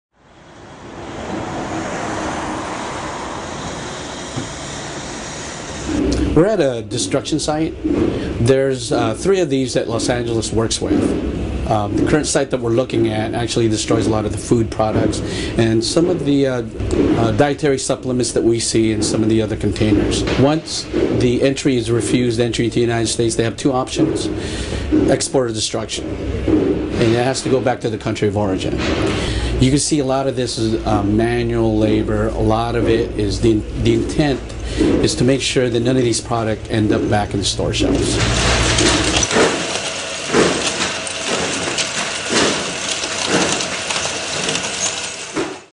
Import Refusals: The Destruction of Unsafe Food
The U.S. Food and Drug Administration physically inspects less than 2 percent of imported food, but denies entry of potentially dangerous products in the thousands. When a commodity is refused entry into the U.S., the company can either export the product or have it destroyed by U.S. regulators. (Video by Kyle Bruggeman and Brad Racino/News21)